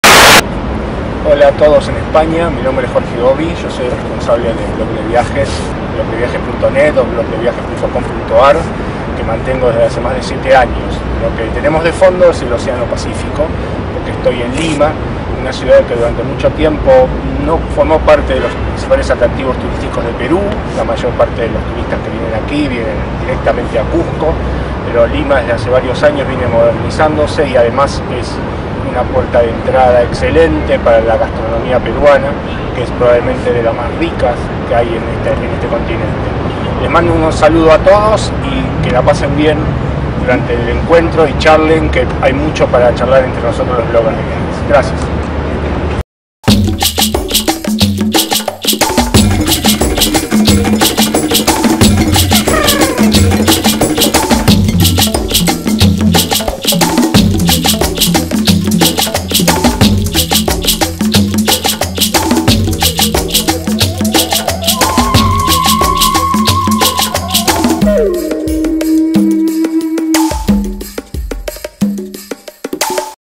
Blogueros en Camino: Perú
Desde Lima (Perú), Jorge Gobbi de nos manda un saludo para el Travel Bloggers Meeting Barcelona 2011 Realización